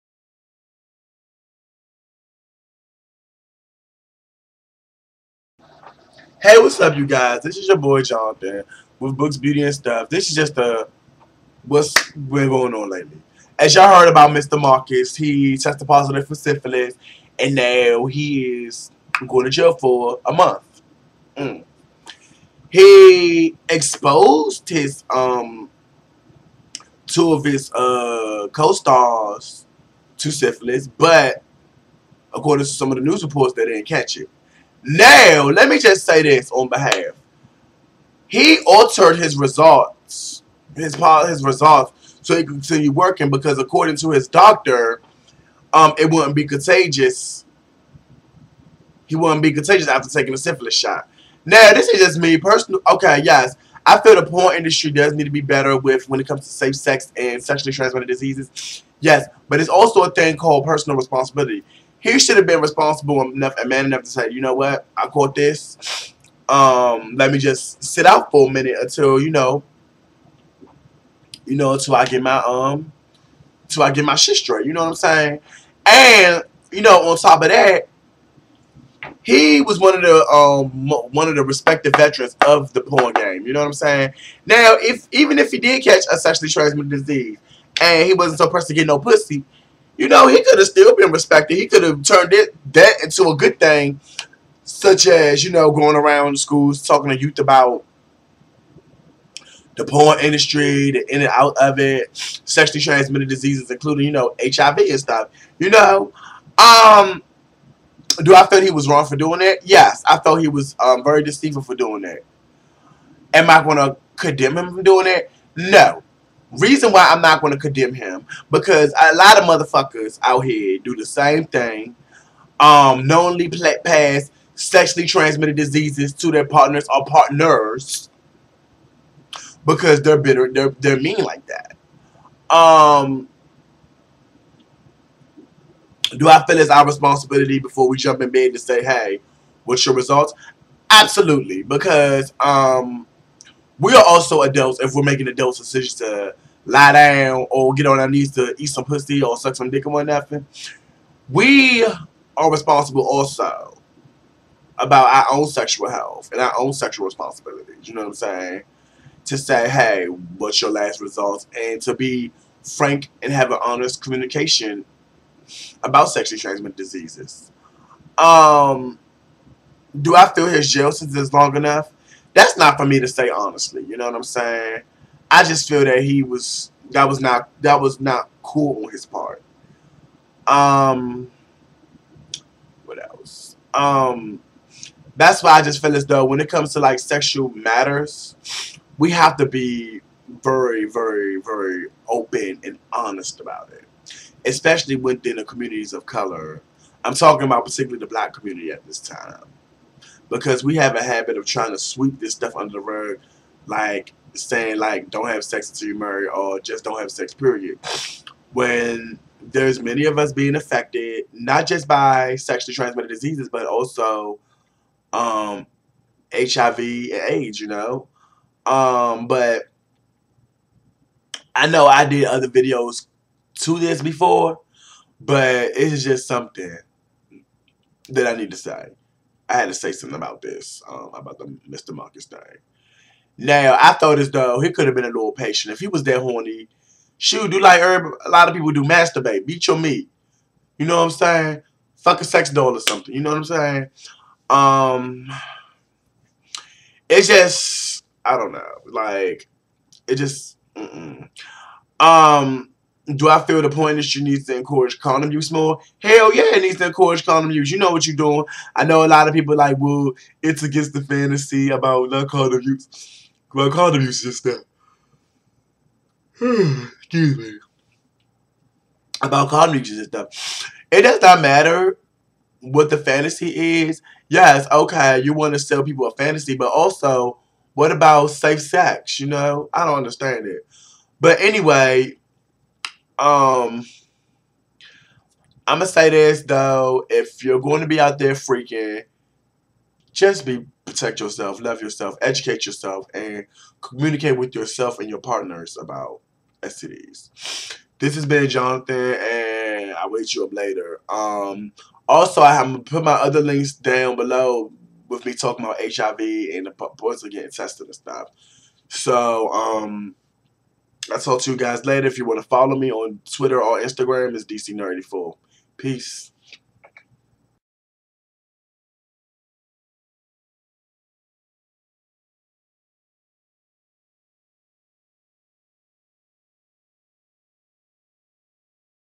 Issues 1-Mr. Marcus Drama
My take on Mr. Marcus's syphilis drama and the porn industry